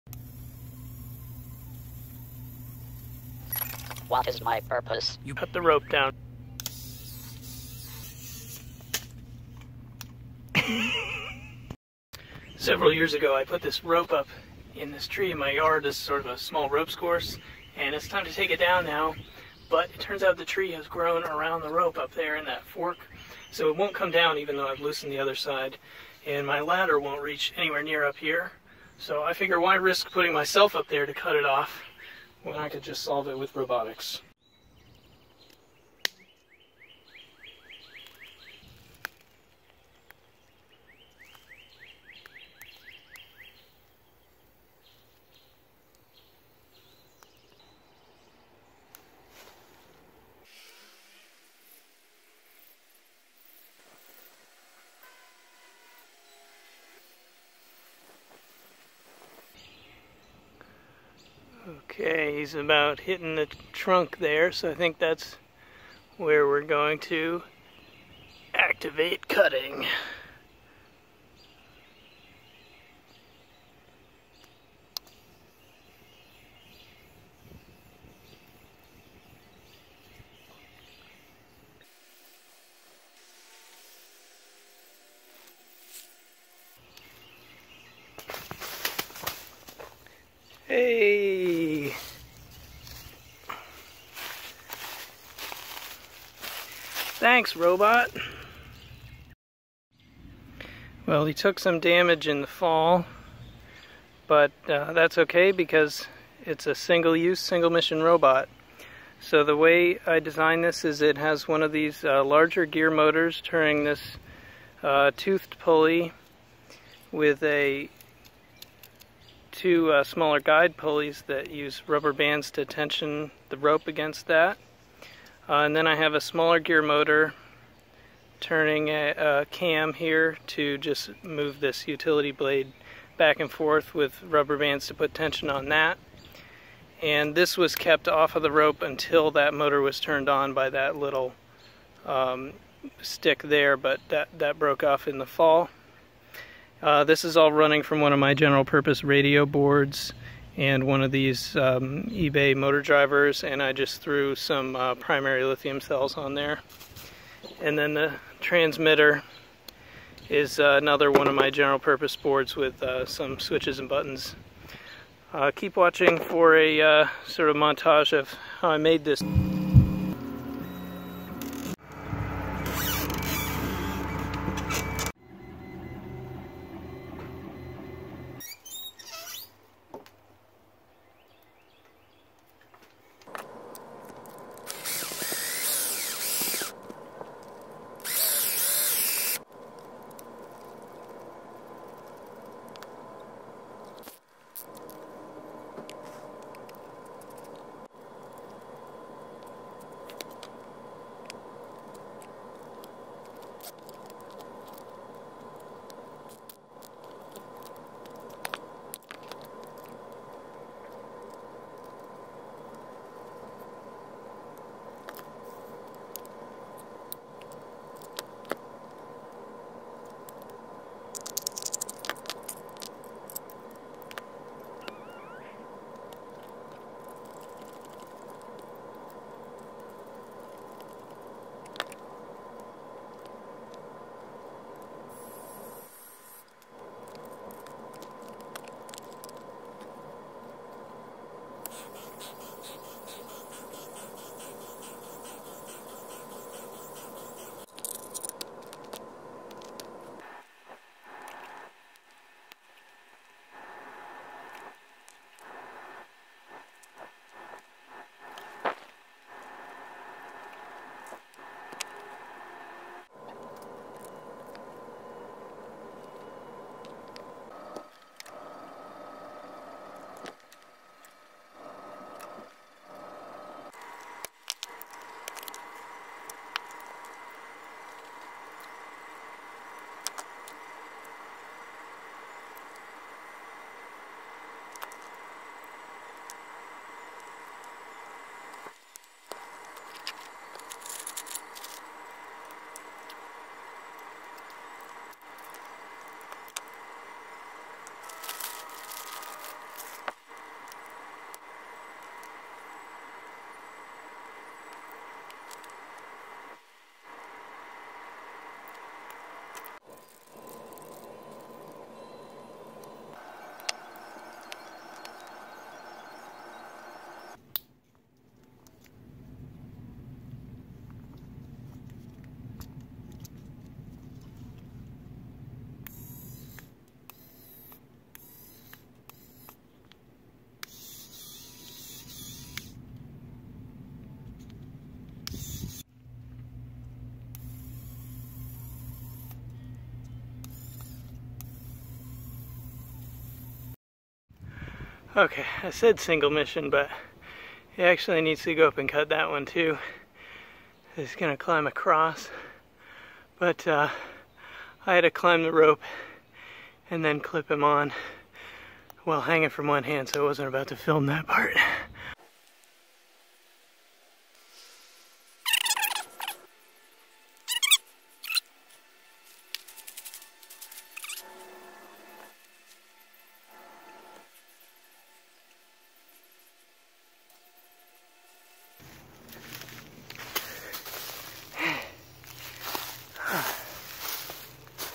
RopeBot! Solving everyday problems with DIY robotics
I 3D-printed a custom robot to accomplish a task that is simple but would have been difficult and dangerous by other means, making this a great application for simple robotics. 3D printers make disposable robotics possible - print a robot for a specific task, assemble from basic building blocks (motors, radio controls, etc.), then break it down and throw it away after the task is complete!